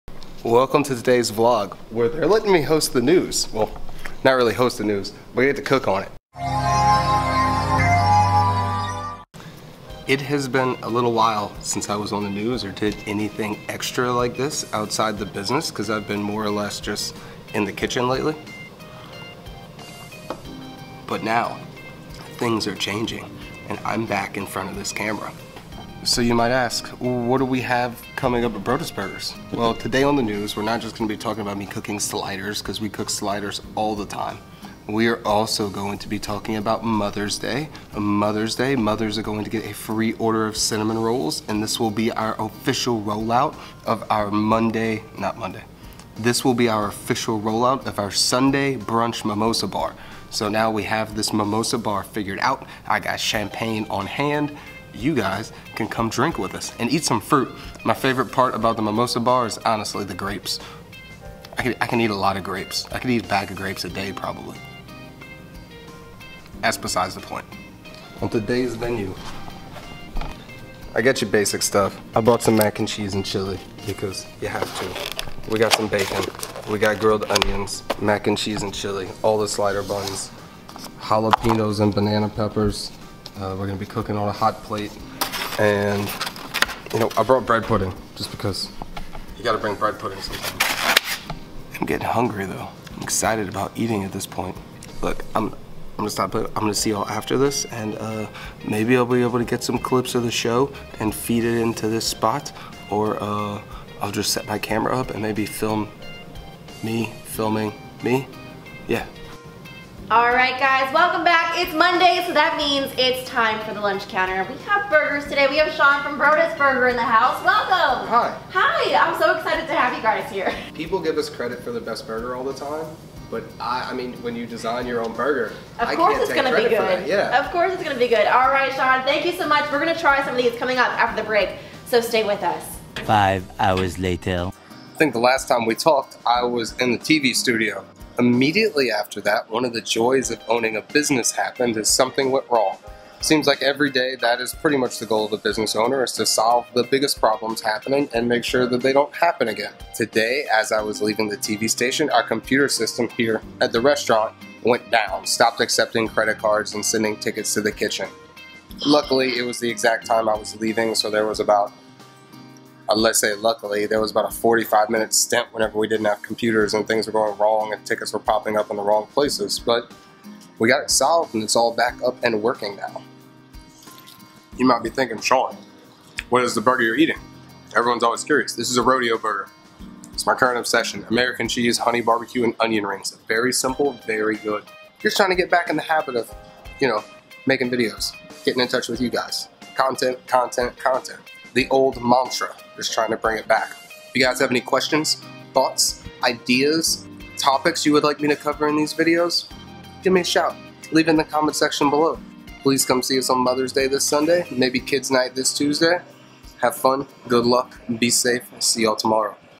Monday May 6 - I''m on the news, moms get free connamon rolls, and we now offer a mimosa bar.
Short vlog from Monday May 6th.